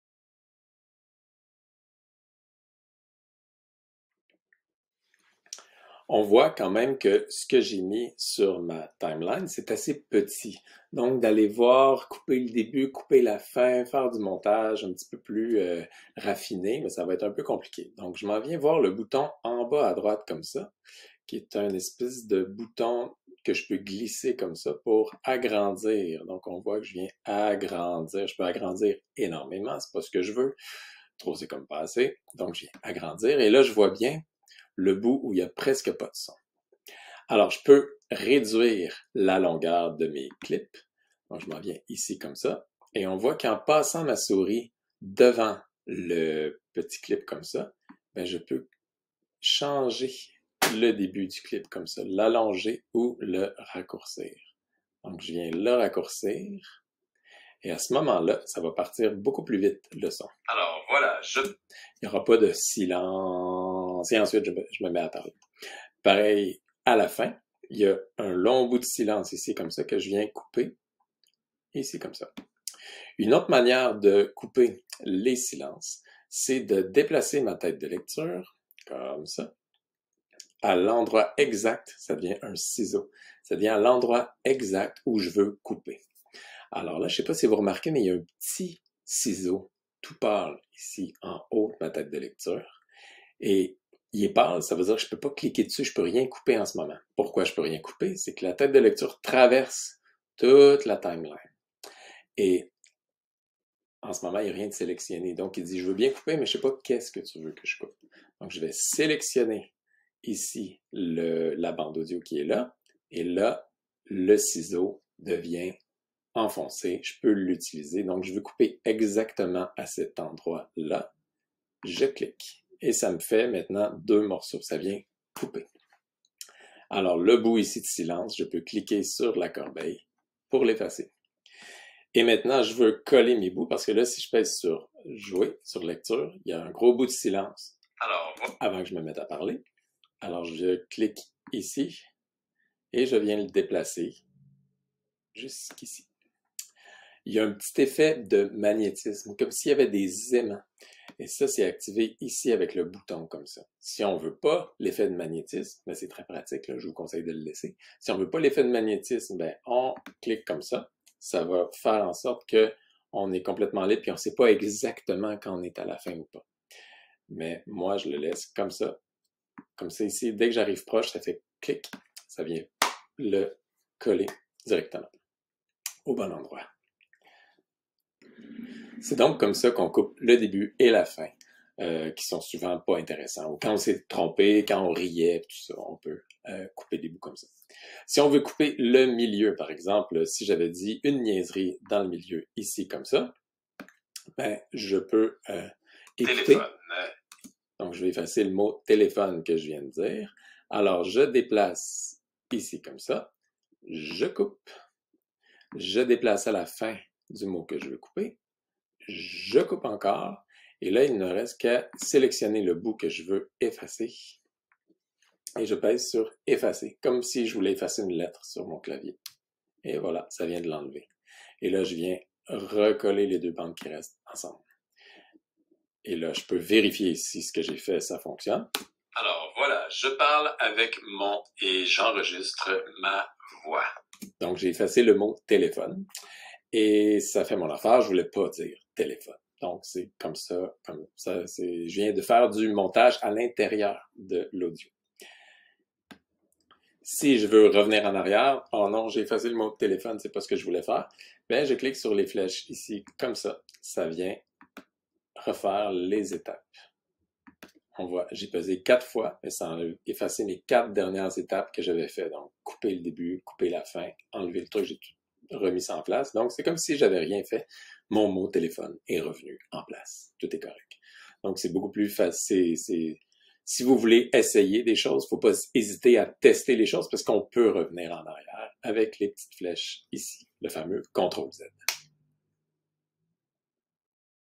Faire du montage dans WeVideo
Dans le cadre d’une création de podcast en classe.